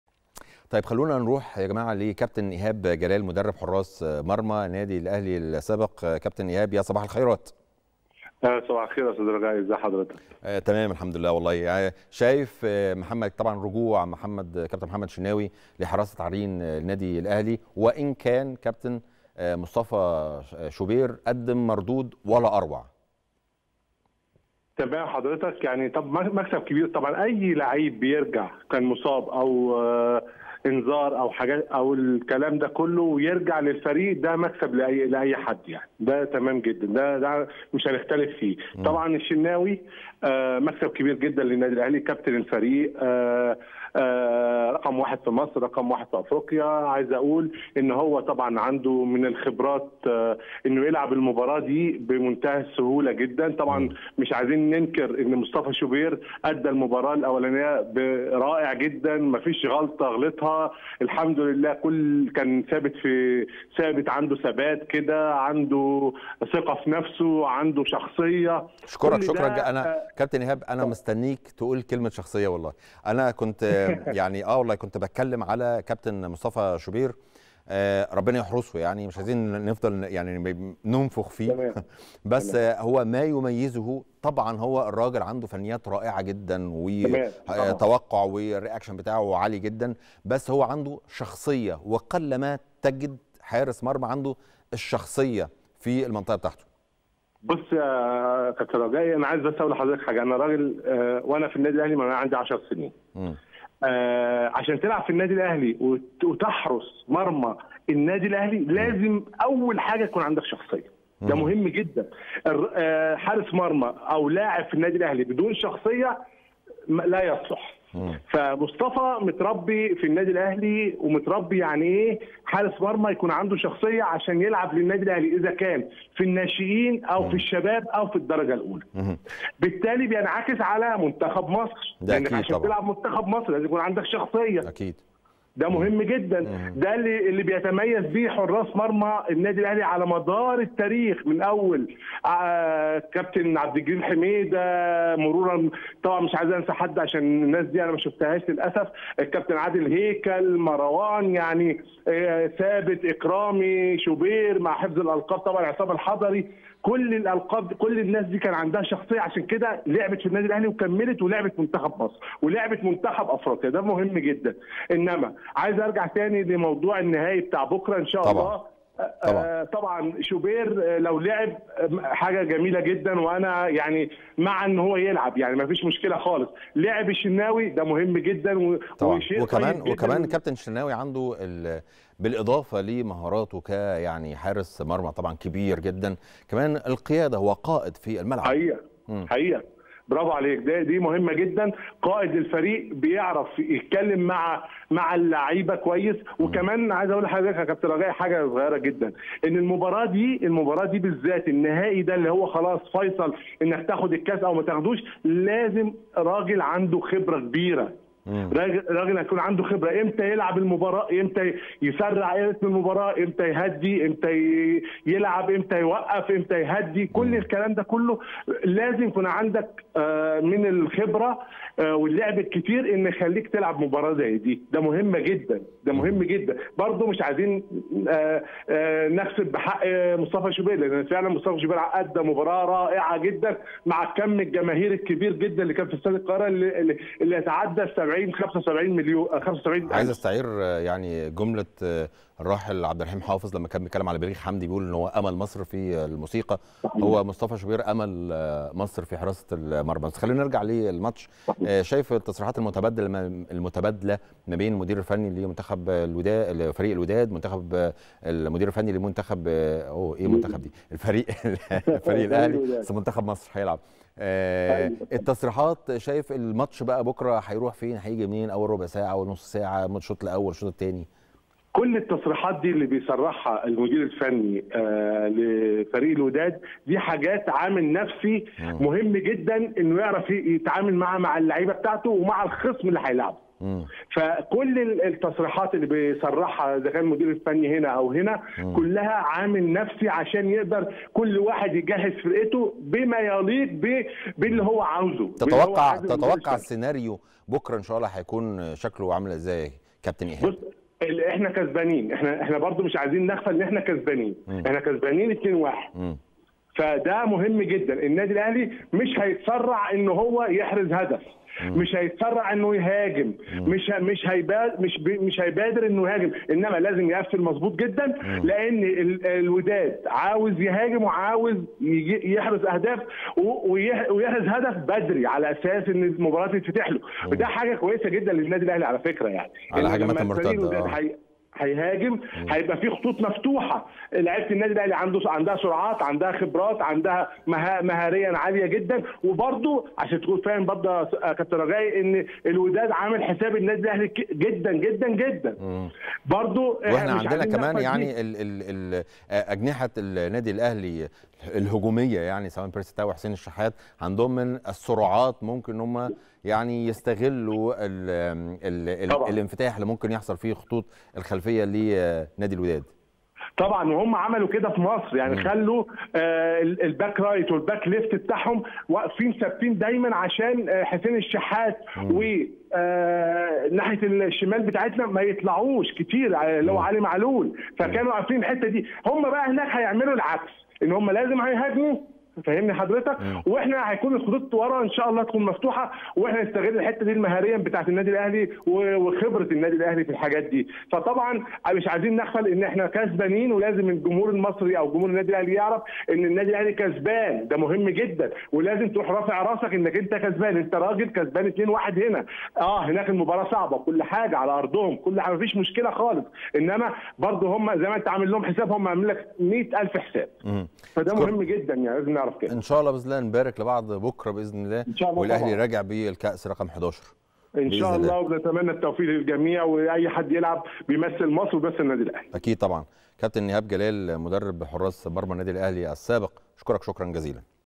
مين أحق بحراسة المرمى الشناوي أم شوبير 🔥ايهاب جلال مدرب حراس مرمى الأهلي الأسبق هيقولك بالتفاصيل 💣👇🏻
مين أحق بحراسة المرمى الشناوي أم شوبير 🔥🔥كابتن ايهاب جلال مدرب حراس مرمى الأهلي الأسبق هيقولك بالتفاصيل 💣👇🏻